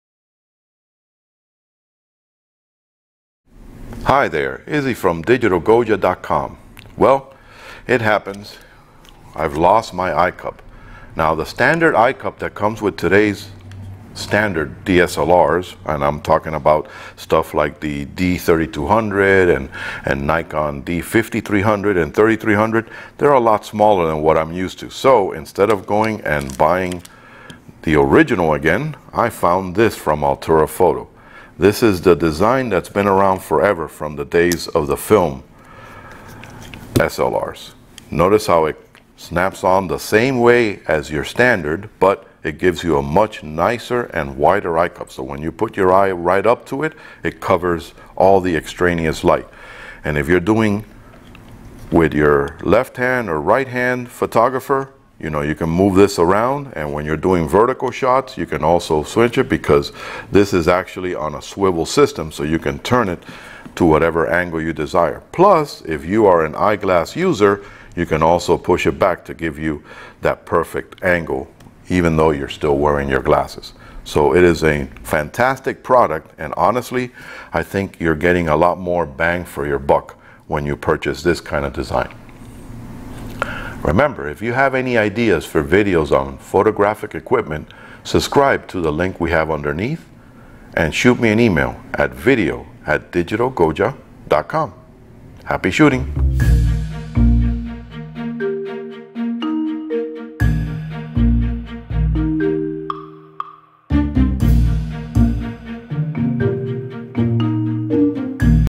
TUTORIAL | How To Use a Camera Eyepiece / Eyecup Replacement for CANON NIKON & Others
Check out this video to learn more about using a replacement camera eyepiece by Altura Photo® Get yours

Features:
This rubber eyepiece links eye to camera for improved viewfinder optics. Easily upgrade your camera eyepiece in seconds. Unique self locking mount system keeps eyecup securely in place.

Compatibilities:

For Nikon:
- Nikon DSLR D7000 D5100 D5000 D3200 D3100 D3000 D90 D8

For canon:

- CANON EOS 1100D, 600D, 550D,500D, 450D, 400D, 300D, 5D, 10D, 20D, 30D, D60, D30
- CANON REBEL T3i, T3, T2i, T1i, XTi, XSi, XS, 1D, 1D MK II